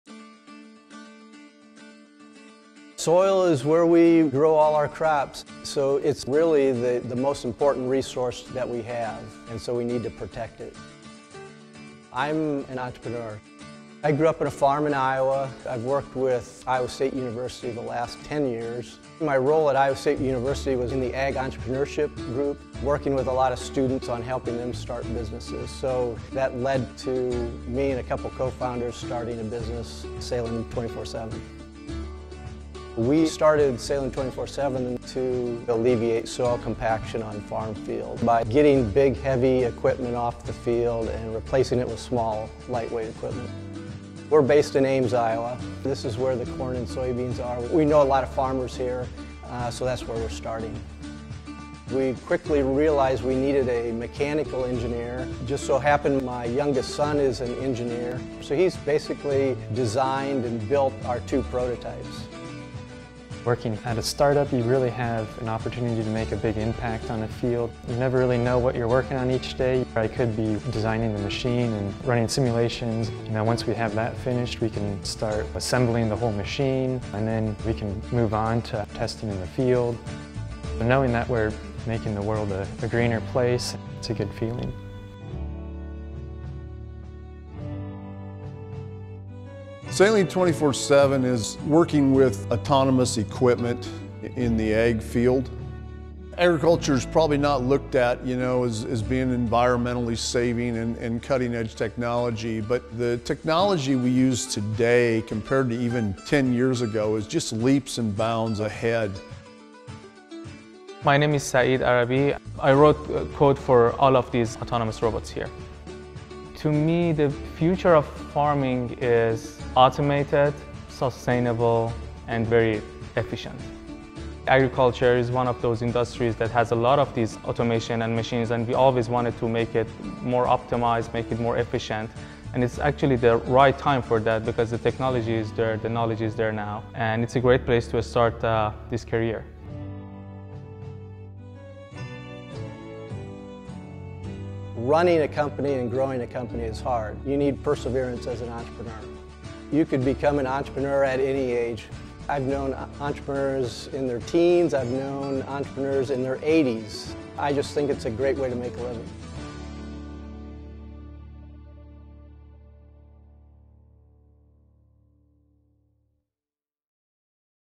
I Want That Jobs!: Robotics & Agriculture
The future of farming is autonomous, sustainable, and highly efficient.

Ben Krog works as a mechanical engineer for Salin 247, a company that designs and manufactures lightweight farming equipment to alleviate soil compaction on crop fields. “You never really know what you’re working on each day; you probably could be designing the machine or running simulations,” says Krog. “Once we have that finished, we can start assembling the whole machine, and then we can move on to testing in the field.” Mechanical engineers like Krog are responsible for developing, designing, building, and testing mechanical devices and sensors.

According to the Bureau of Labor Statistics (BLS), the median annual wage for mechanical engineers was $95,300 in May 2021. Employment of software developers is projected to grow 2 percent from 2021 to 2031. Despite limited employment growth, about 17,900 openings for mechanical engineers are projected each year, on average, over the next decade. Most of those openings are expected to result from the need to replace workers who transfer to different occupations or exit the labor force, such as to retire. Mechanical engineers typically need a bachelor’s degree in mechanical engineering or mechanical engineering technology.

Saeed Arabi works as a software engineer for Salin 247. “Agriculture is one of those industries that has a lot of these automation and machines, and we always wanted to make it more optimized, more efficient,” says Arabi. “It’s a great place to start this career.” Software engineers like Arabi design computer applications or programs, and many work in manufacturing, computer systems design, or for software publishers. They often work on teams with other software developers, as well as with quality assurance analysts and testers.

The median annual wage for software engineers is $110,140. Employment of software engineers is projected to grow 25 percent from 2021 to 2031, according to the BLS, which is much faster than the average for all occupations. About 162,900 openings for software developers, quality assurance analysts, and testers are projected each year on average over the decade. Many of those openings are expected to result from the need to replace workers who transfer to different occupations or exit the labor force, such as to retire. Software engineers typically need a bachelor’s degree in computer and information technology or a related field, and some employers prefer to hire developers who have a master’s degree.

Watch all the I Want That Job! videos here.

Credits:
• Featuring: David Krog, Ben Krog, Doyle Stern,
• Producers: George Freund, Melissa Panzer
• Executive Producers: Melissa Panzer, Joan Lynch, Art Bilger
• Field Producer:  Mike Vogel
• Editor: George Freund
• Color Correction: Jerimiah Morey
• Associate Producer: Diana Aydin
• Illustrator and Graphic Designer: Daniele Simonelli
• Motion Designer: Matteo Goi

For stories about solutions to the jobs skills gap disrupting our economy and the future of work, visit WorkingNation